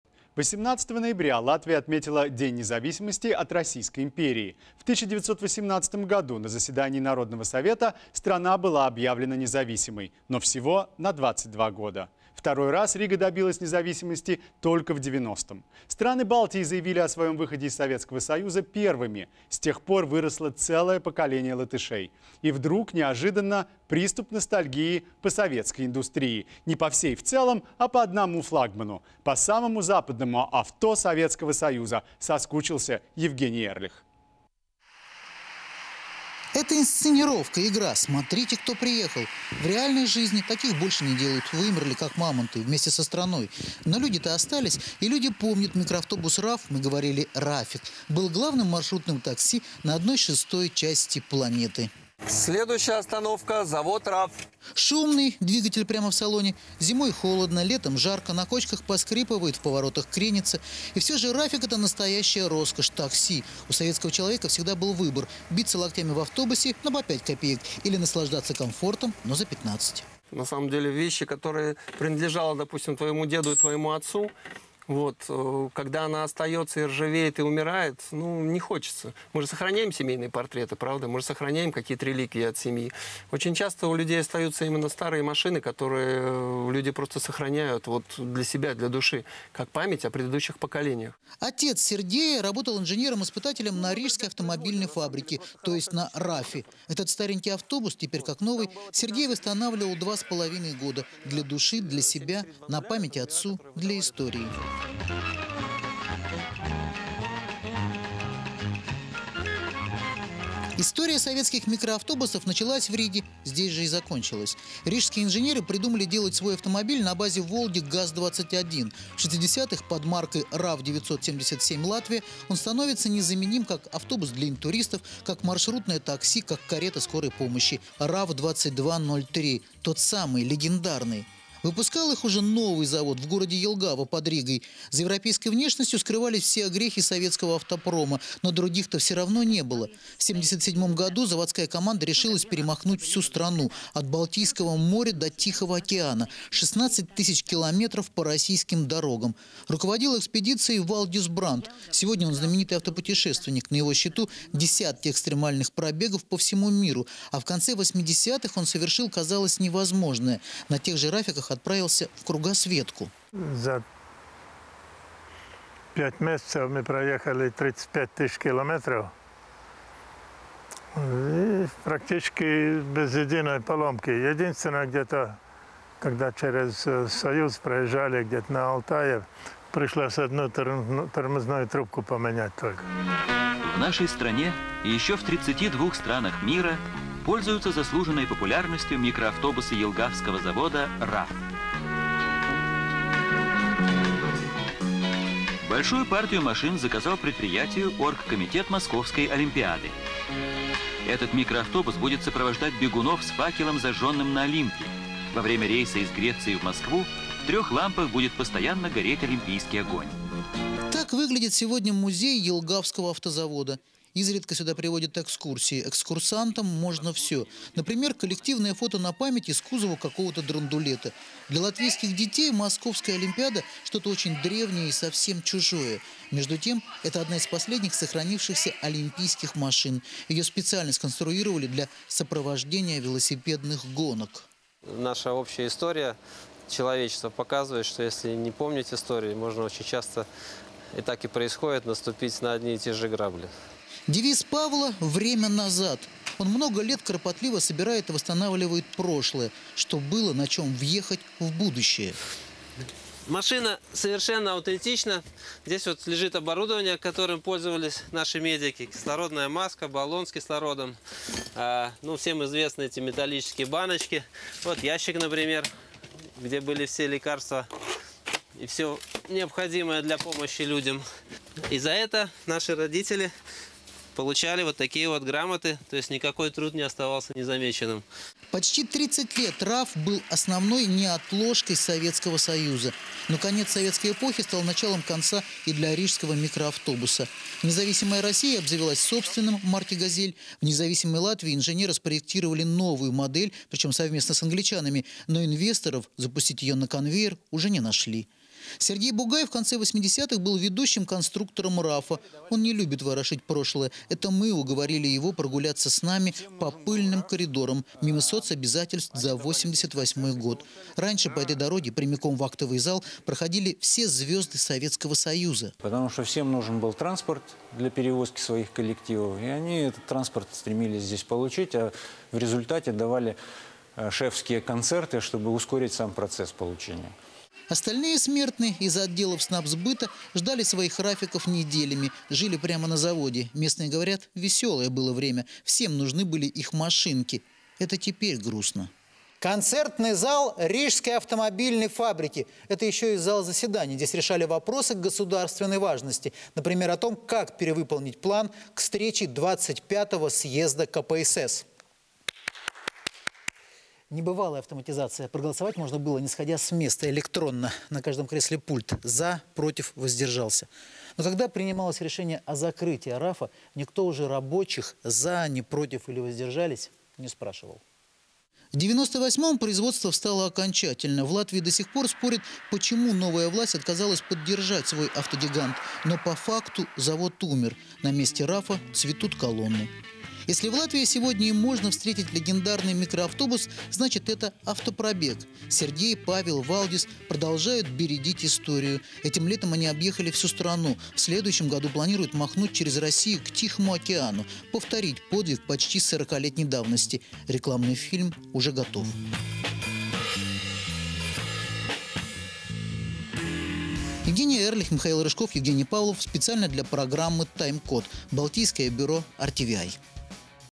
Латвия: приступ ностальгии по советскому автопрому
Страны Балтии заявили о своем выходе из Советского Союза первыми, с тех пор выросло целое поколение латышей. И вдруг неожиданно – приступ ностальгии по советской индустрии. Не по всей в целом, а по одному флагману. По самому западному авто Советского Союза соскучился Евгений Эрлих.